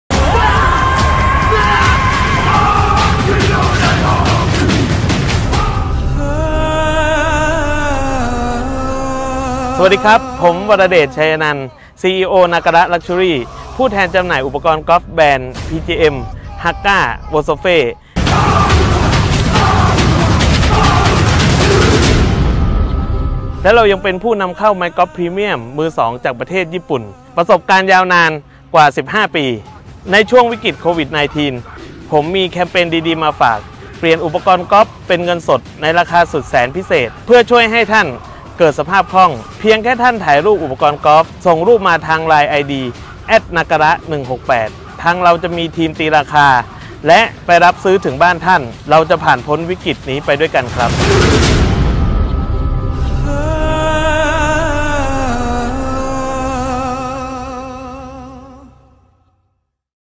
" วรเดช " ชวน ร่วมแคมเปญ NakaraLuxurious
" วรเดช " นำทัพ ดารานักแสดง เข้าร่วมแคมเปญ NakaraLuxurious
คุณ วรเดช ไชยนันทน์ CEO NakaraLuxurious ร่วมกับ ดารานักแสดง จากละครเรื่อง บุพเพนาคราช และ สูตรลับฉบับวังนาคี ผุดแคมเปญ NakaraLuxurious
              ในช่วงวิกฤต โควิด - 19 เราทุกคนต่างช่วยกันคนละไม้ละมือ ในส่วนที่ตนเองสามารถทำได้เพื่อช่วยทุกคนในสังคมของเราผ่านพ้นวิกฤตนี้ไปด้วยกัน
NakaraLuxurious Co.,Ltd ได้คิดแคมเปญดีๆ รับซื้อไม้กอล์ฟ ในราคาพิเศษที่สุด เพื่อช่วยให้ท่านเกิดสภาพคล่องทางการเงิน และใช้ช่องทางจำหน่ายทั้งหมดที่มีของ NakaraLuxurious ในการช่วยระบายสินค้า
ท่านสามารถส่งรูปอุปกรณ์กอล์ฟของท่าน โดยการกดลิงค์ จะมีทีมงานตีราคา และเดินทางไปรับซื้อถึงหน้าบ้านของท่าน
             โฆษณาความยาว 1 นาที ได้ประชาสัมพันธ์ต่อเนื่อง ทางช่อง MCOT HD และหวังเป็นอย่างยิ่งว่าทุกๆท่านจะช่วยกันแชร์วีดีโอนี้ ส่งต่อสิ่งดีๆร่วมกันครับ 💙